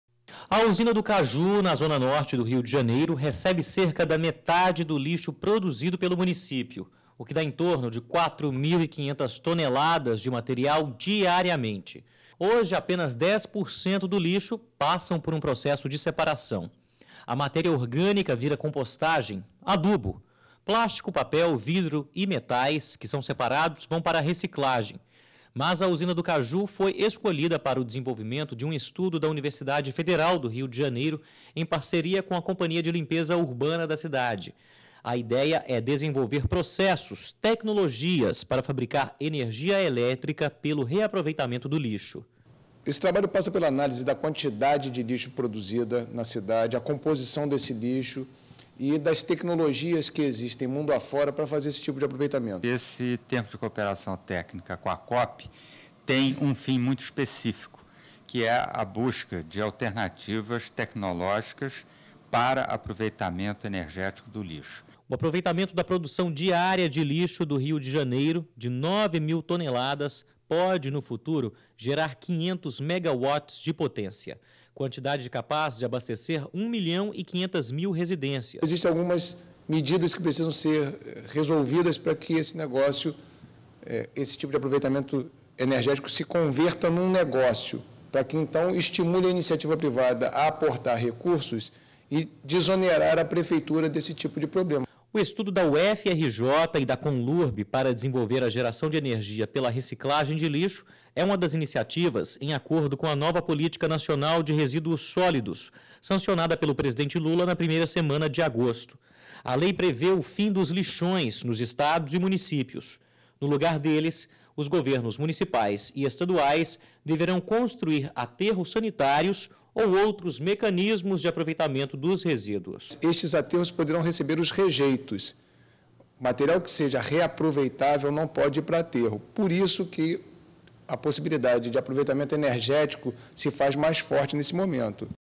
Estudo mostra que os lixões são os principais destinos dos resíduos
E o lixo pode virar energia elétrica. Pesquisa realizada no Rio de Janeiro vai avaliar as condições para implantar usinas de geração de energia a partir dos resíduos.